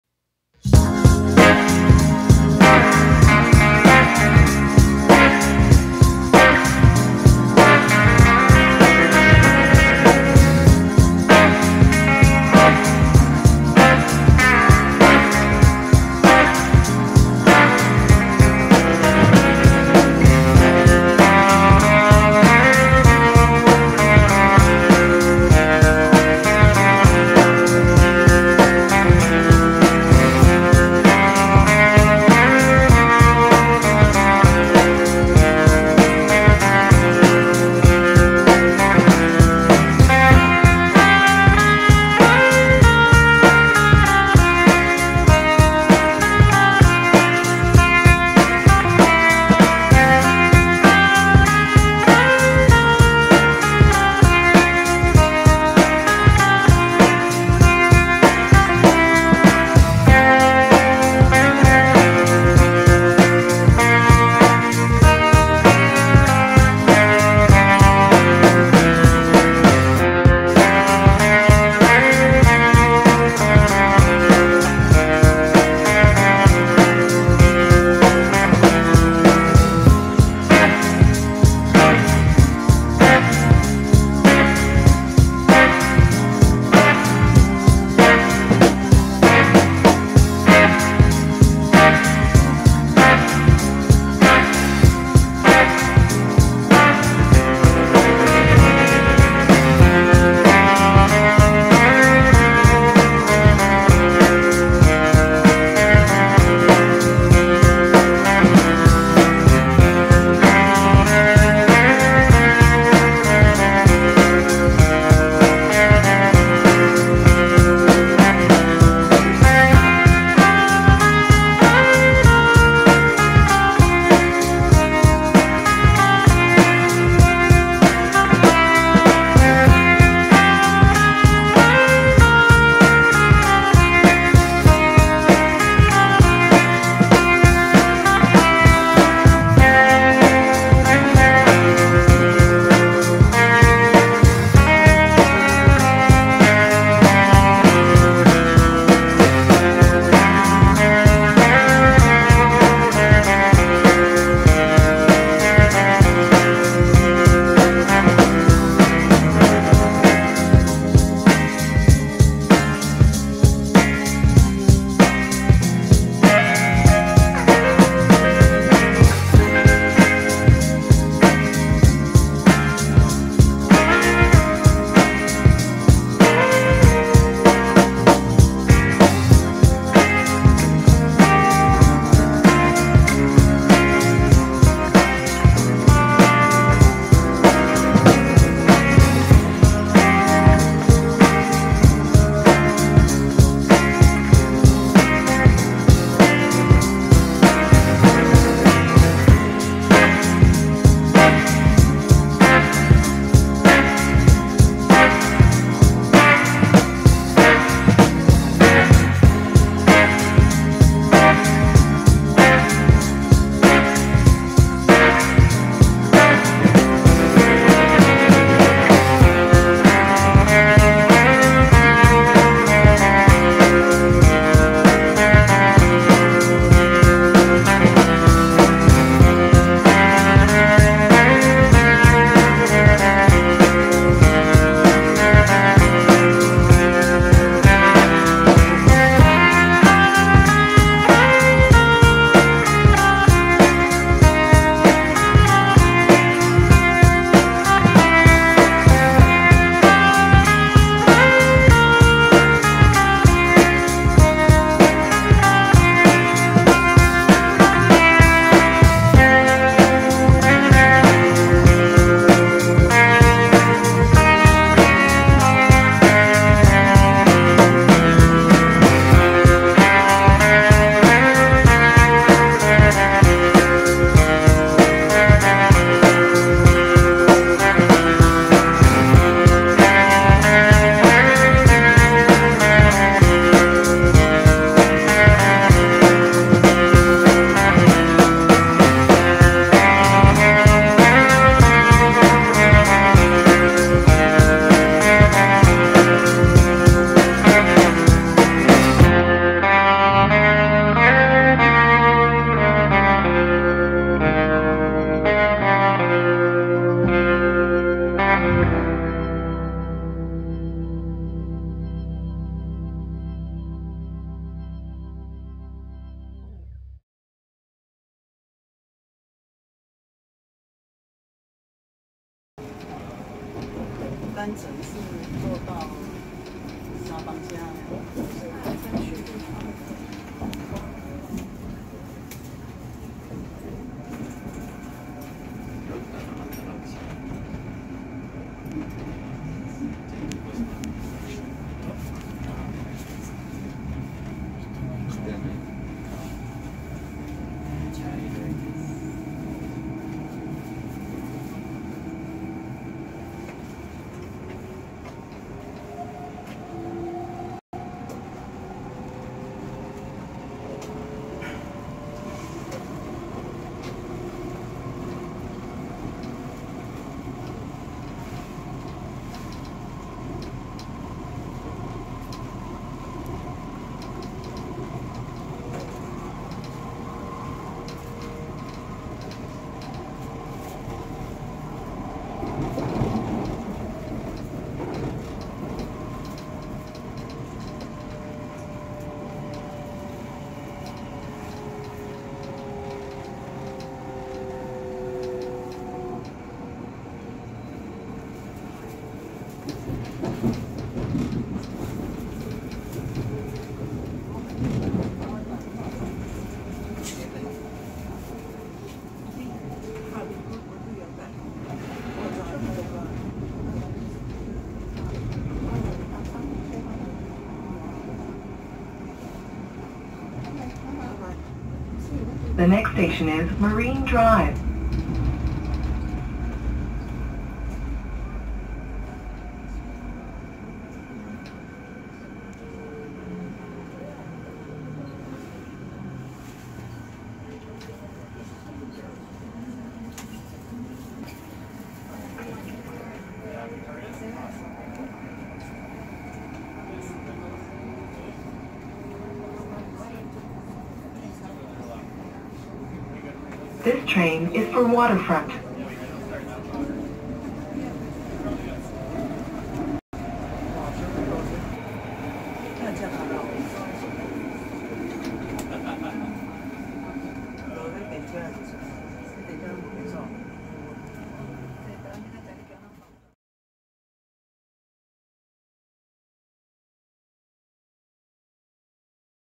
Vancouver C-train Line Ride
Going from Richmond to downtown area.